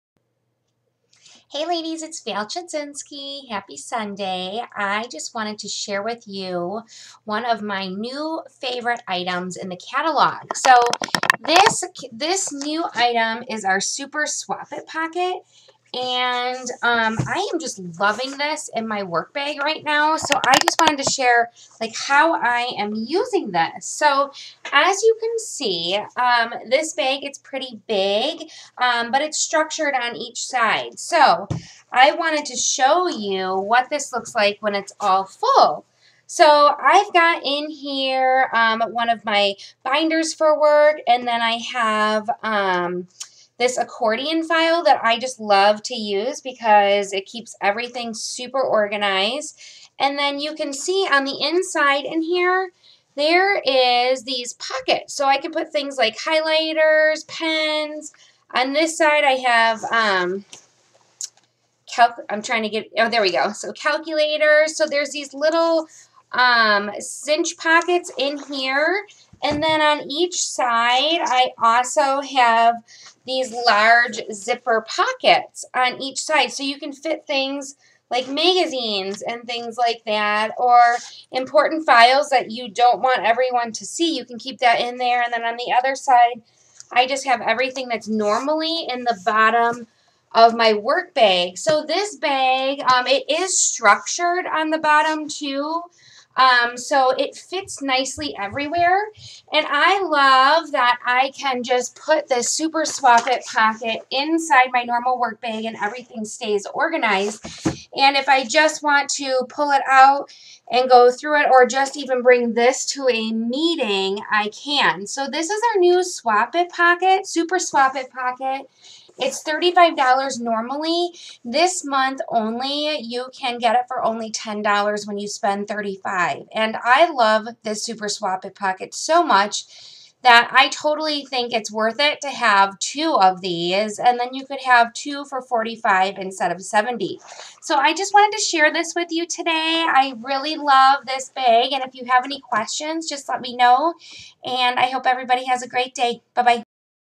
Super Swap It Pocket - Val Chudzinski
LOVE this bag for work!  Check out how this organizes my work bag!  Want it or want to see the colors it comes in?  Check it out